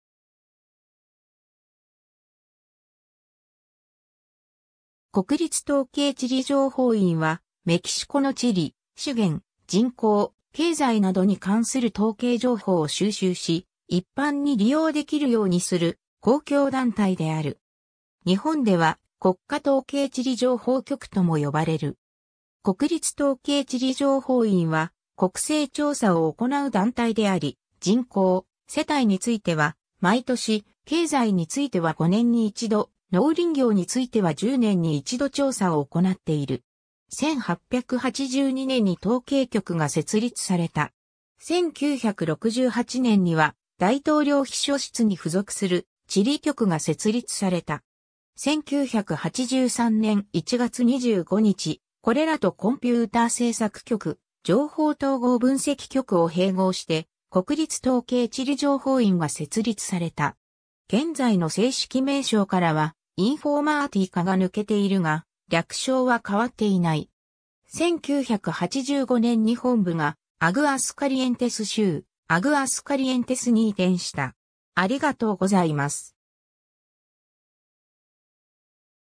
国立統計地理情報院
国立統計地理情報院（こくりつとうけいちりじょうほういん、スペイン語: Instituto Nacional de Estadística y Geografía, INEGI）は、メキシコの地理・資源・人口・経済などに関する統計情報を収集し、一般に利用できるようにする公共団体である。
日本では国家統計地理情報局とも呼ばれる。
 国立統計地理情報院は国勢調査を行う団体であり、人口・世帯については毎年、経済については5年に一度、農林業については10年に一度調査を行っている。
 1882年に統計局(Dirección General de Estadística)が設立された。
1968年には大統領秘書室に附属する地理局(Dirección General de Geografía)が設立された。
1983年1月25日、これらとコンピューター政策局、情報統合分析局を併合して国立統計地理情報院(Instituto Nacional de Estadística, Geografía e Informática)が設立された。
現在の正式名称からは「Informática」が抜けているが、略称は変わっていない。
 1985年に本部がアグアスカリエンテス州アグアスカリエンテスに移転した。